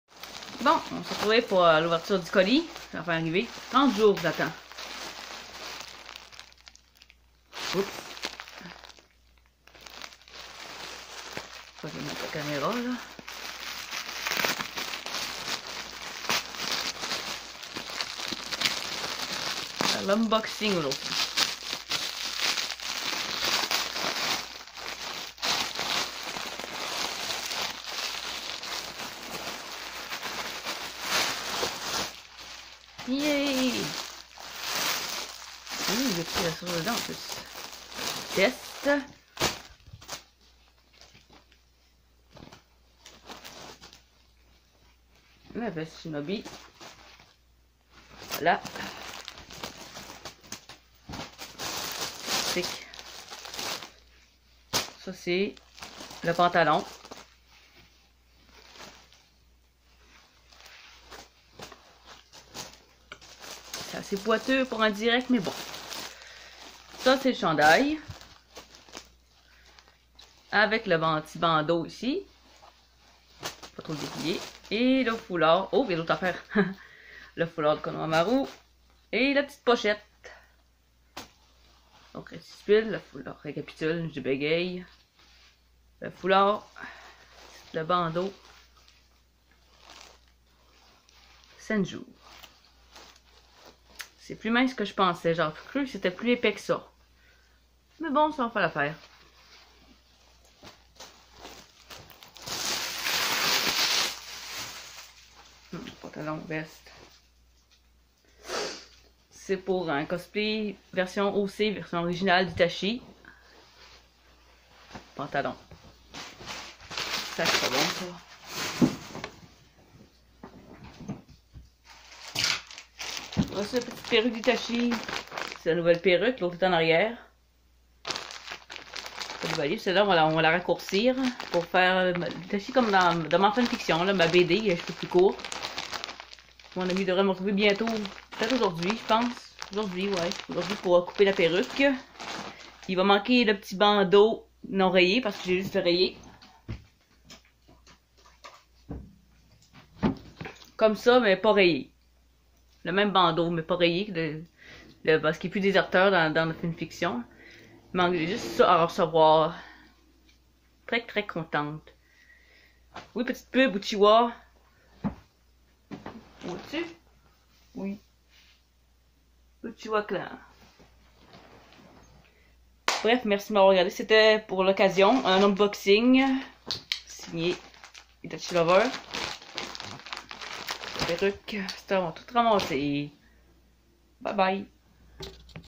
Konohamaru cosplay onboxing
Après 30 jours d'attente, ça fait exactement 1 mois jour pour jour que j'ai acheté ce cosplay.
J'ouvre le colis devant vous.

▬▬▬▬▬▬▬▬▬▬▬▬ VIDÉOS ASSOCIÉES  ▬▬▬▬▬▬▬▬▬▬▬

Karasu Cosplay

Karasu cosplay